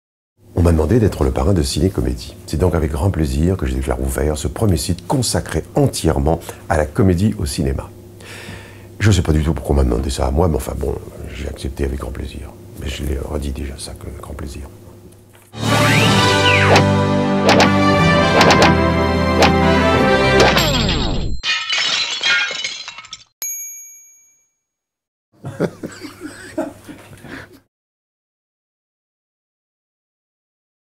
Pierre Richard, parrain de CineComedies
Pierre Richard est le parrain de CineComedies, premier site entièrement consacré à la comédie au cinéma.